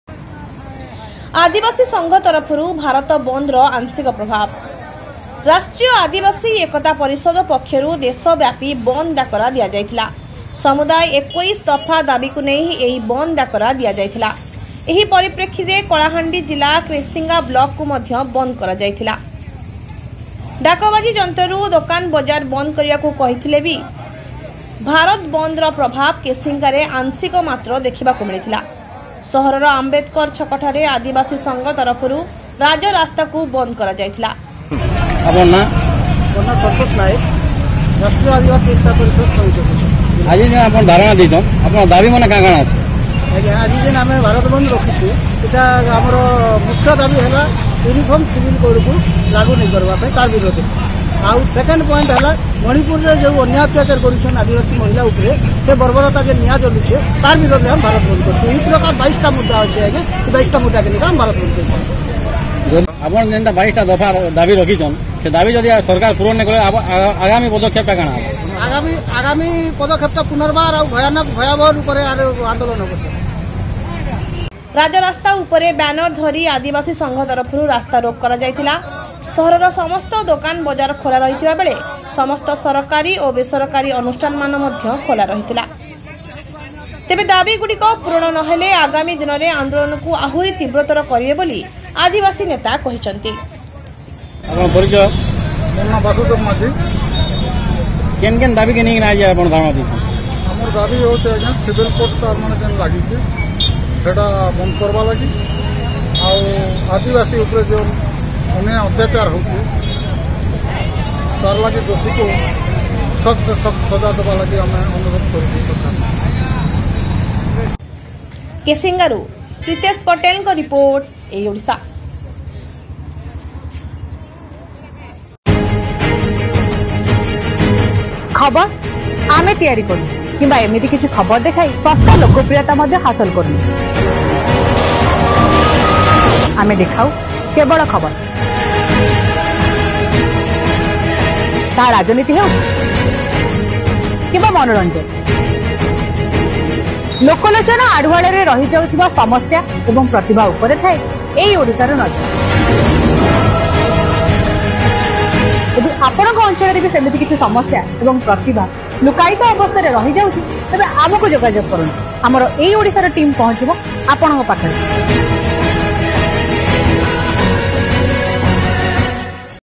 କଳାହାଣ୍ଡି ଖବର II KALAHANDI NEWS II AEI ODISHA II ଆଦିବାସୀ ସଂଘ ତରଫରୁ ଭାରତ ବନ୍ଦ ର ଆଂଶିକ ପ୍ରଭାବ II
odisha news
odisha news today
odisha news room
odishanews 2023
odisha news aaj ka
odisha news khabar
     Follow on us: